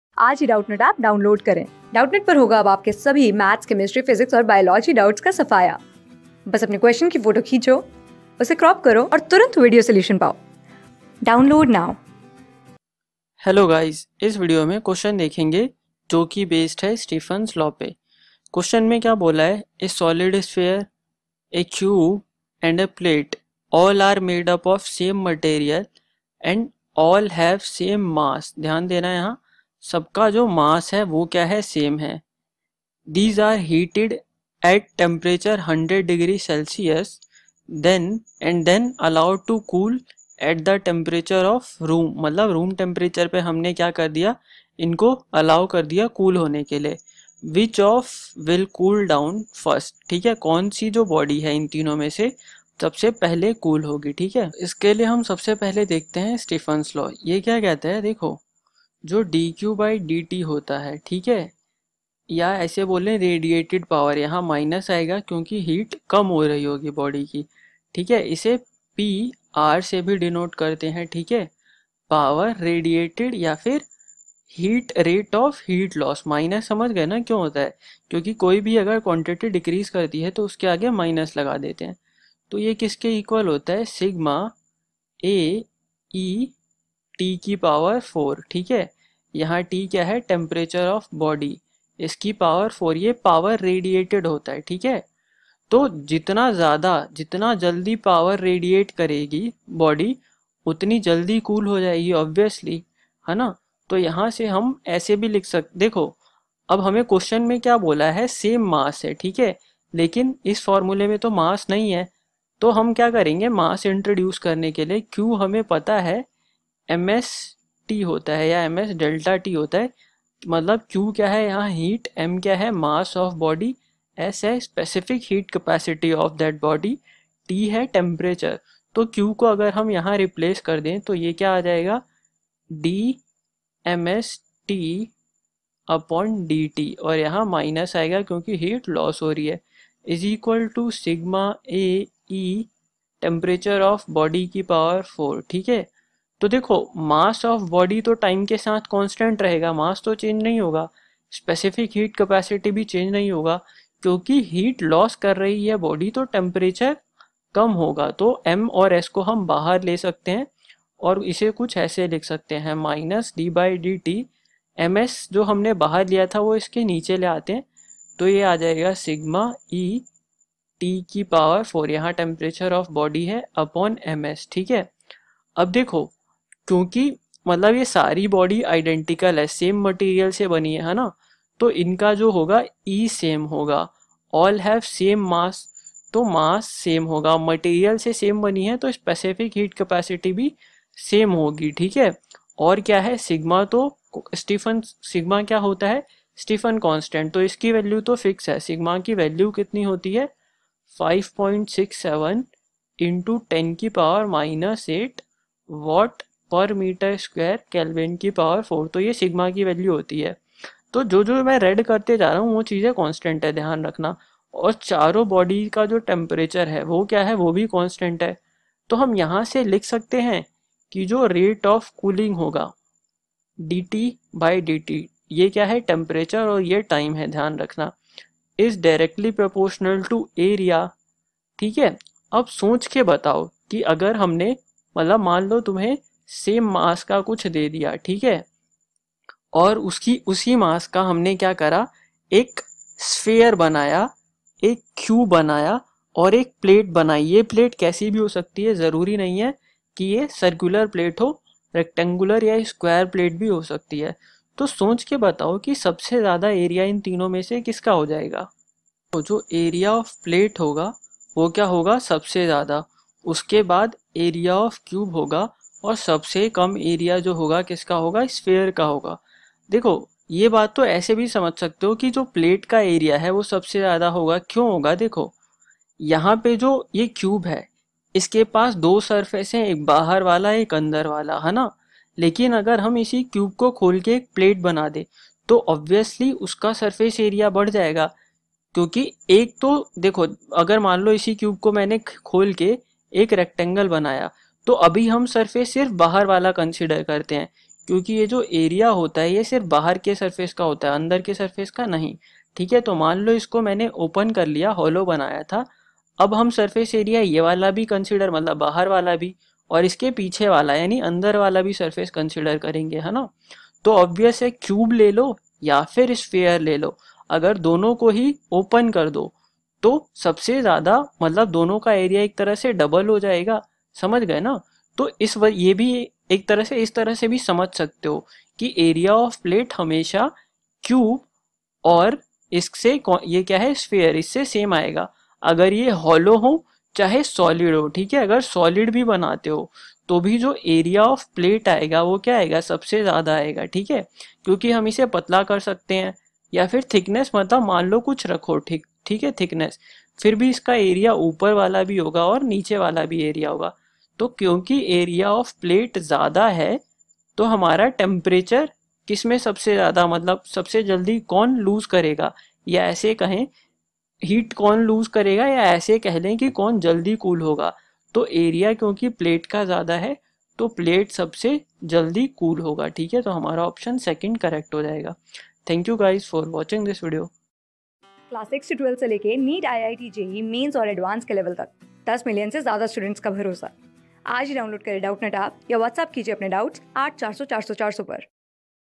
A solid sphere, a cube and a plate, all are made of same material and all have same mass. These ...
A solid sphere, a cube and a plate, all are made of same material and all have same mass. These are heated to a temperature 100^@C and then allowed to cool at the temperature of room. Which of these will cool down first –
Class: 12
Subject: PHYSICS
Chapter: HEAT TRANSFER & THERMAL EXPANSION
Board:IIT JEE

👉 Have Any Query? Ask Us.